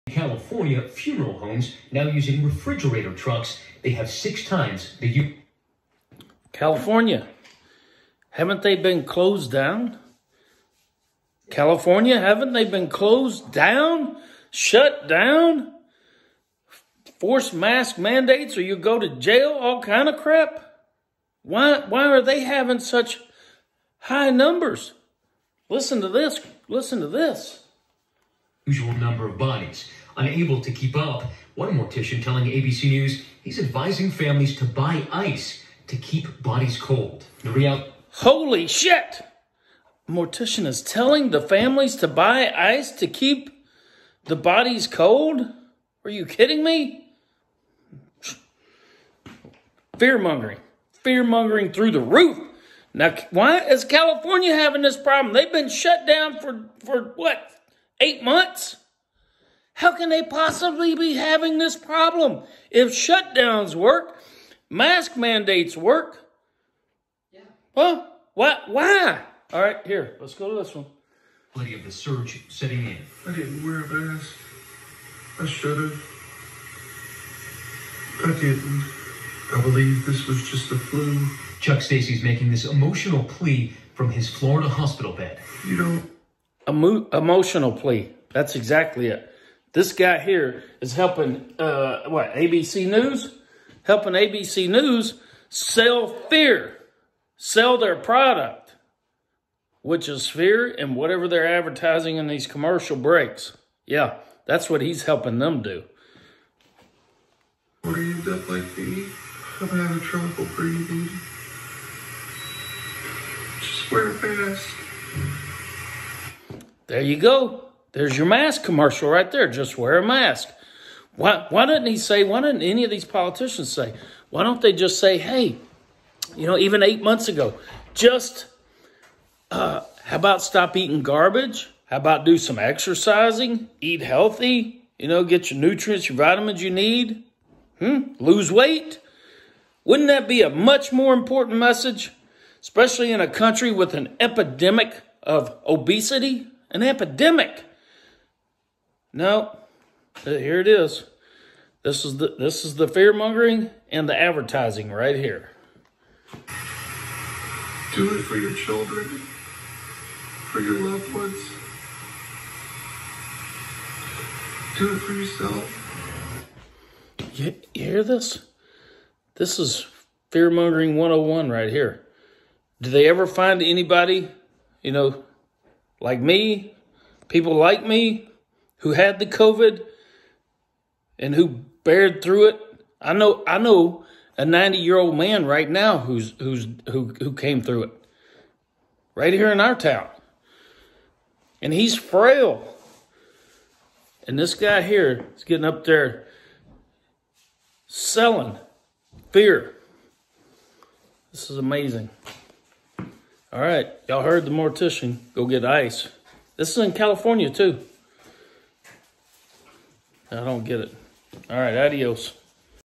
ABC corporate “news” fear mongering through the roof! Unbelievable Bullshit!
Florida is not locked down, California is locked down, does that make any fucking sense?